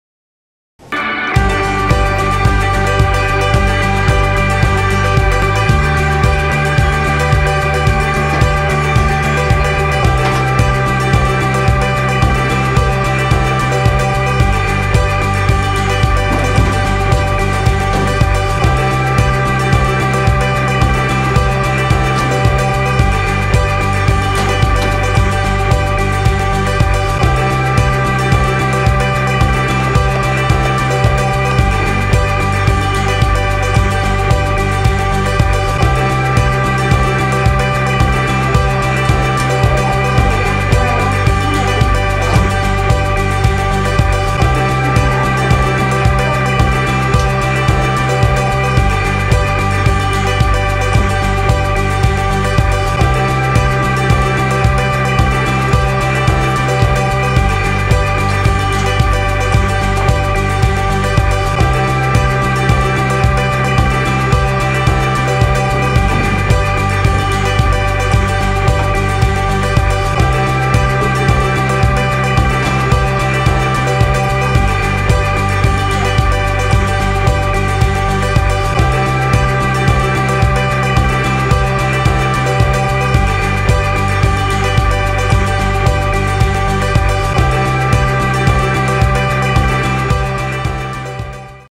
PhotoBoothNE.com 1 min 30 sec set up!
Chris from the PhotoBoothNE.com Team sets up this photo booth in 1 min 30 seconds!